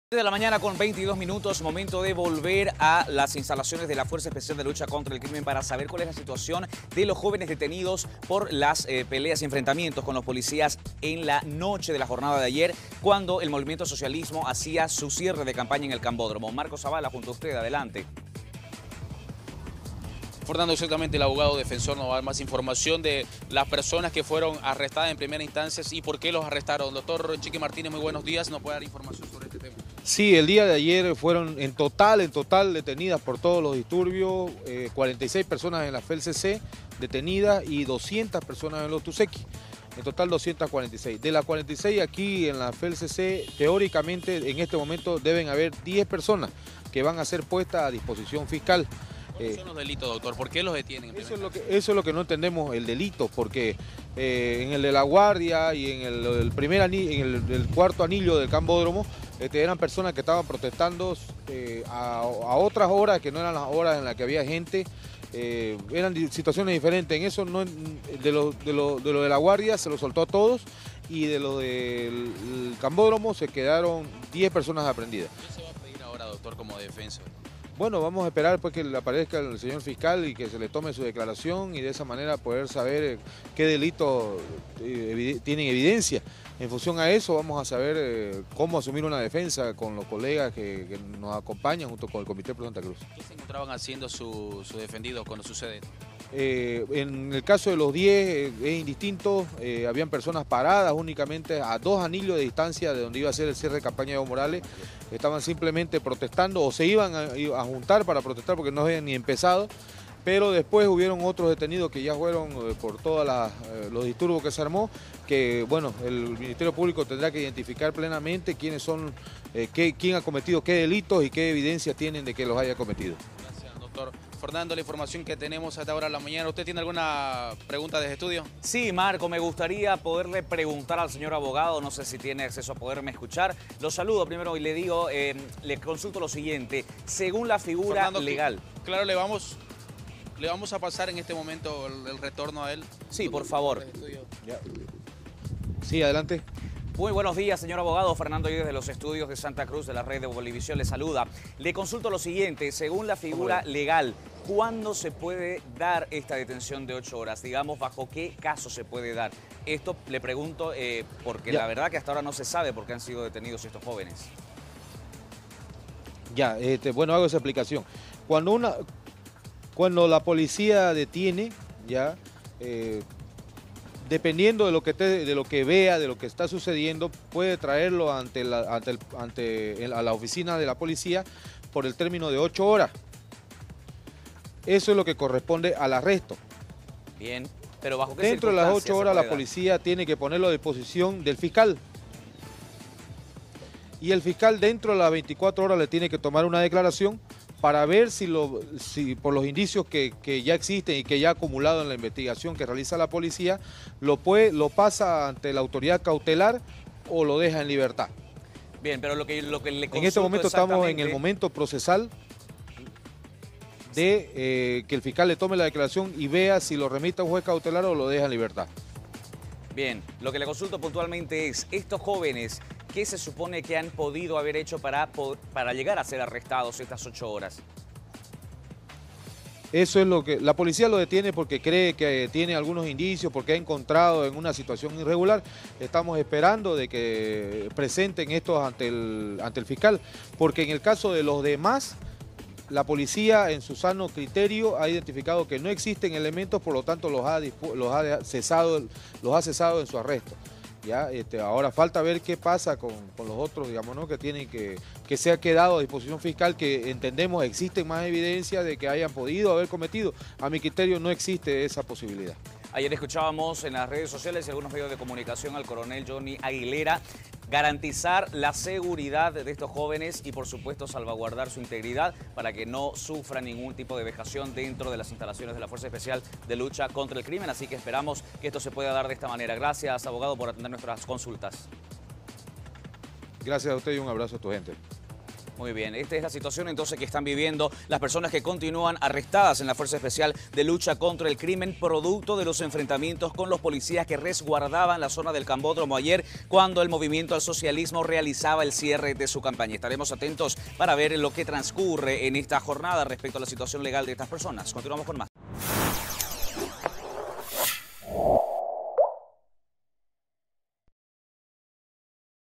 10 arrestados por conflictos en cierre de campaña del MAS
En un completo informe desde Santa Cruz le mostramos los detalles de las detenciones realizadas por los conflictos registrados en el cierre de campaña del Movimiento Al Socialismo.